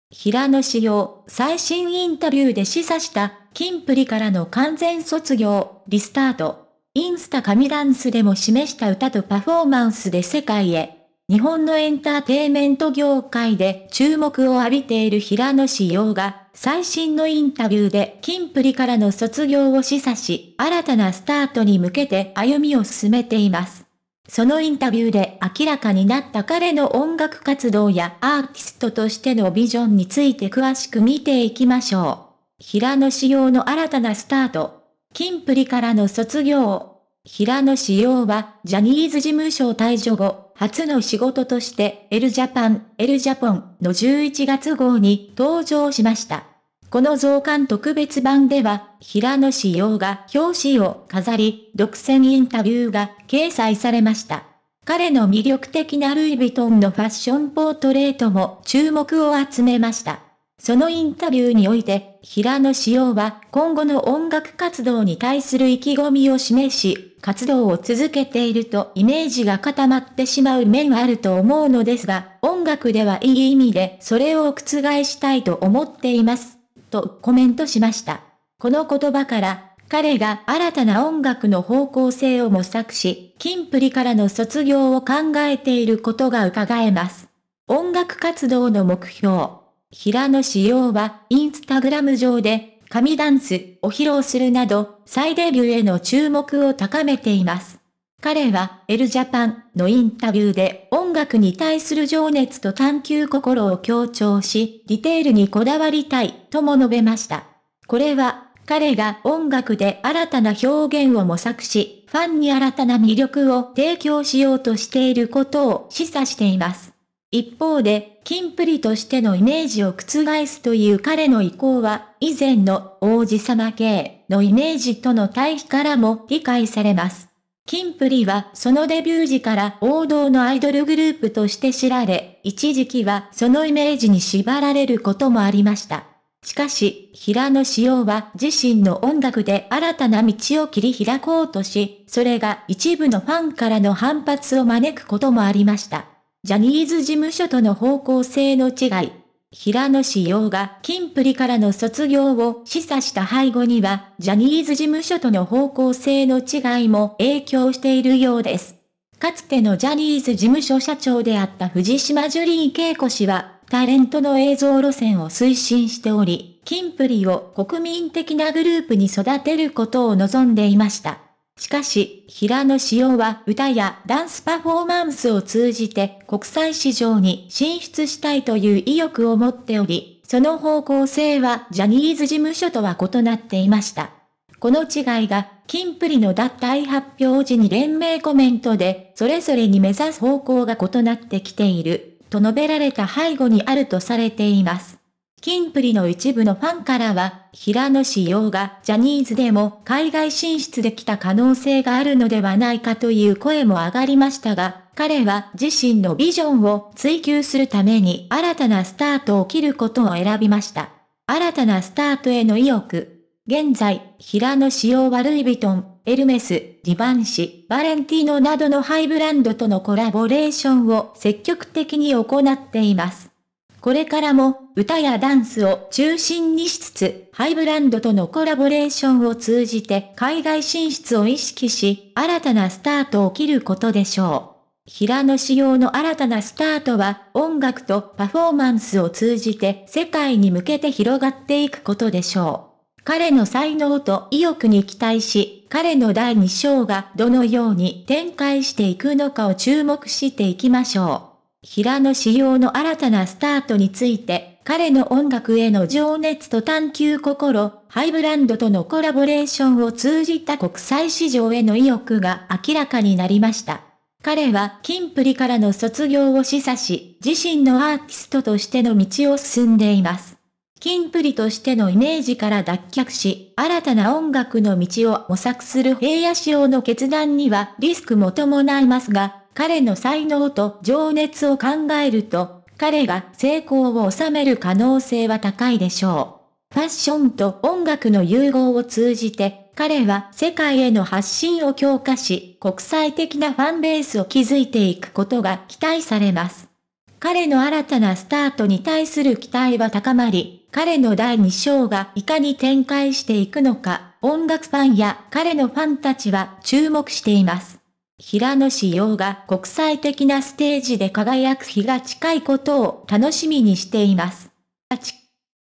【平野紫耀】平野紫耀、最新インタビューで示唆した「キンプリからの完全卒業」リスタート！「インスタ神ダンス」でも示した歌とパフォーマンスで世界へ!!
【平野紫耀】平野紫耀、最新インタビューで示唆した「キンプリからの完全卒業」リスタート！